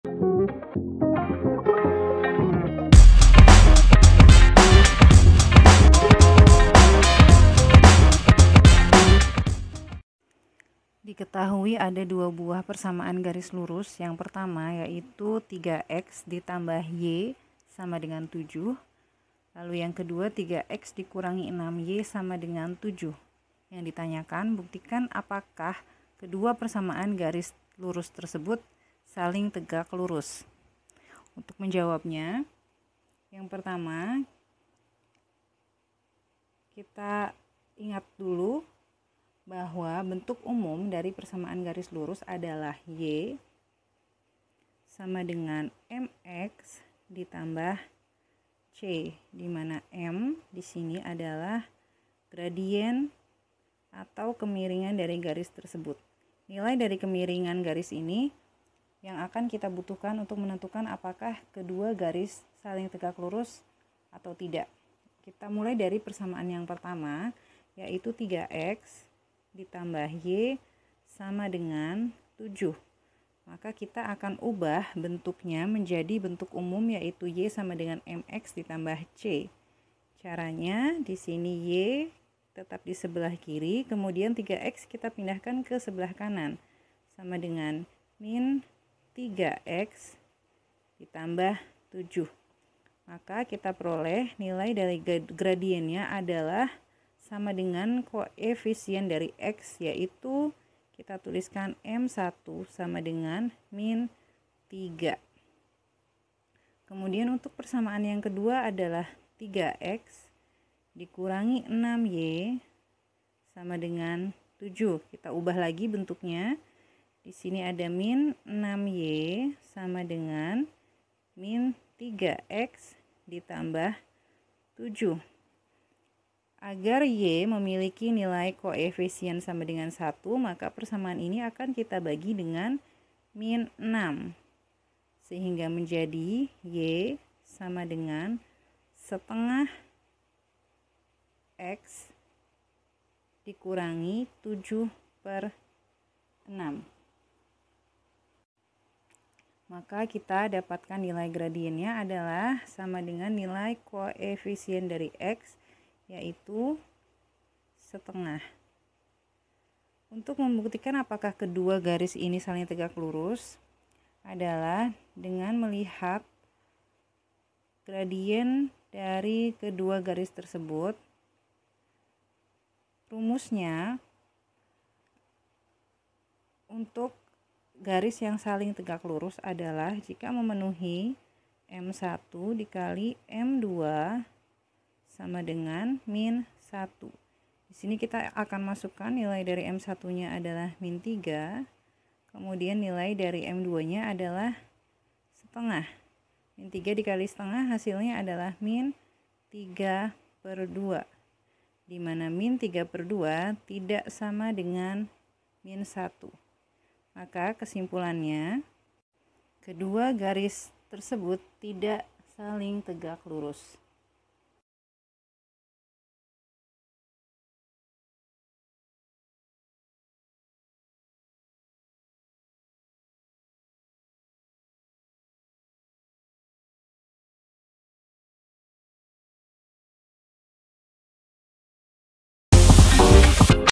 Coba buktikan apakah persamaan garis lurus... | Matematika SMP/ | Kelas 8 | 4
✅ Dalam video ini kita akan membahas: Coba buktikan apakah persamaan garis lurus berikut saling tegak lurus.
b. 3x + y = 7 dengan 3x – 6y = 7
Tema:
Kelas: 8
Mata Pelajaran: Matematika
Bab: 4
Buku: Matematika SMP/MTs Kelas VIII Semester 2

👉 Buku yang telah tersedia:
{PLAYLIST_LINK}
Di mana kamu dapat menemukan kami?